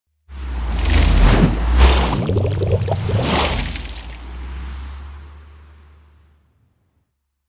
Drawing sounds for games in Unity
Using DSP Motion to draw sounds for your games in Unity.